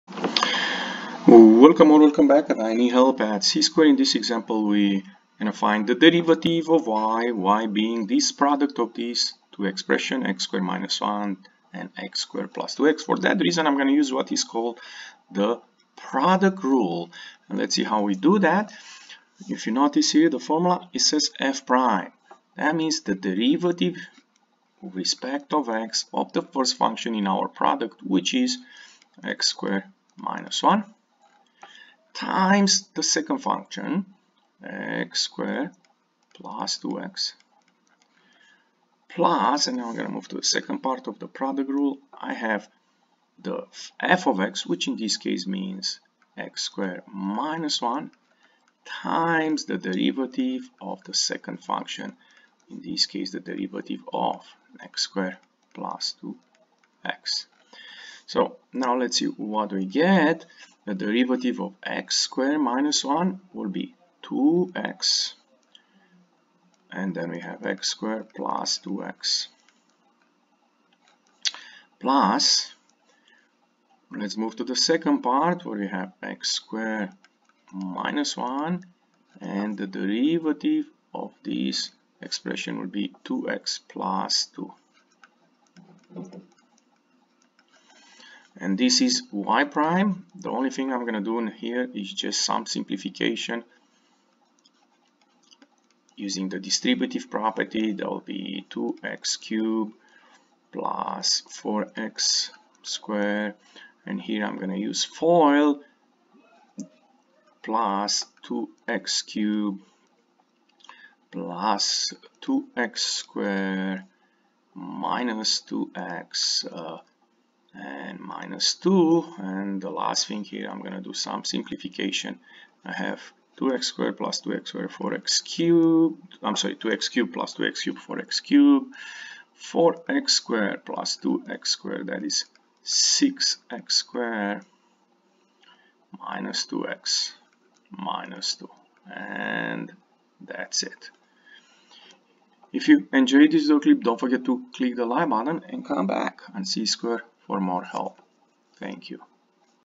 INH: Product Rules #1
This is an example of using the product rule of differentiation.